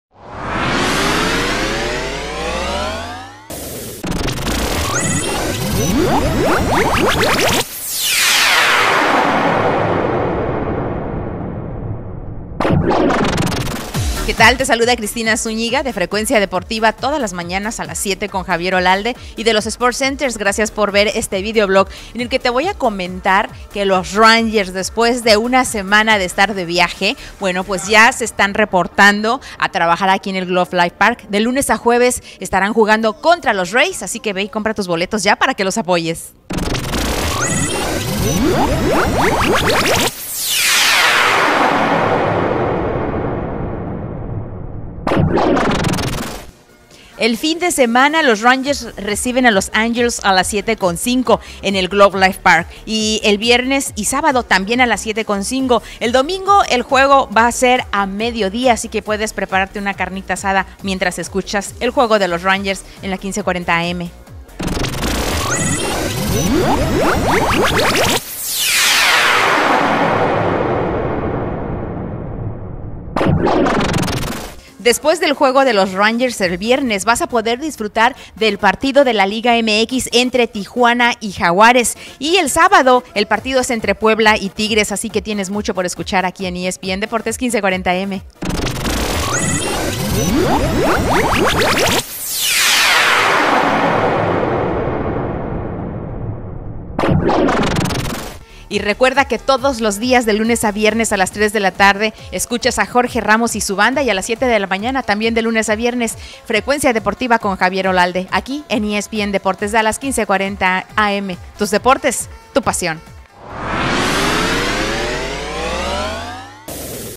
Cristina Zuniga nos tiene un adelanto de lo que estaremos viviendo para la siguiente semana en ESPN
Cristina Zuniga nos tiene un adelanto de lo que estaremos viviendo para la siguiente semana en ESPN Deportes Dallas 1540 AM.